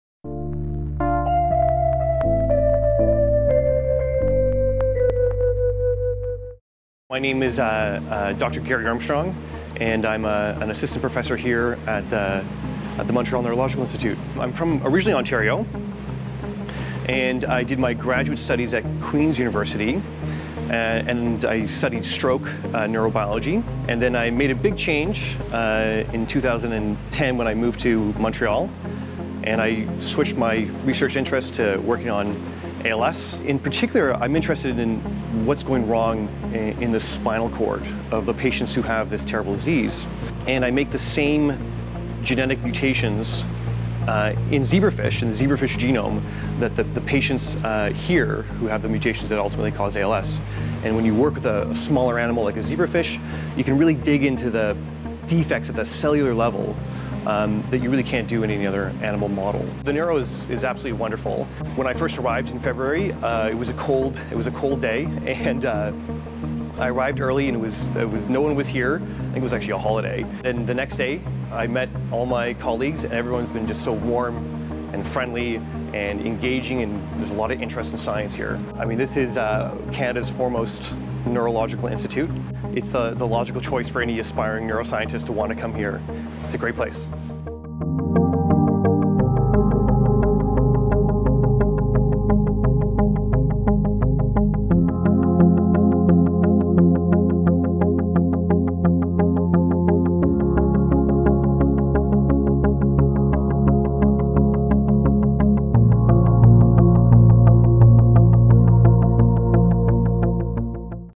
Gary Armstrong - The spinal cord and ALS
Gary Armstrong considers The Neuro to be Canada’s foremost neurological centre.

He cites The Neuro’s many Canadian “firsts,” including the introduction of the electroencephalogram, many of the first magnetic resonance images, and a host of research and clinical techniques such as the Montreal procedure for epilepsy.

“It’s the logical choice for any aspiring neuroscientist to want to come here,” says Armstrong.

As a graduate student in Ontario, he studied stroke neurobiology, but changed his research direction in Montreal, where he became interested in amyotrophic lateral sclerosis (ALS).

“I’m particularly interested in what’s going wrong at the cellular level in the spinal cord of patients with ALS. The better we can understand defects that arise there, the quicker we can advance therapeutic discovery” he says.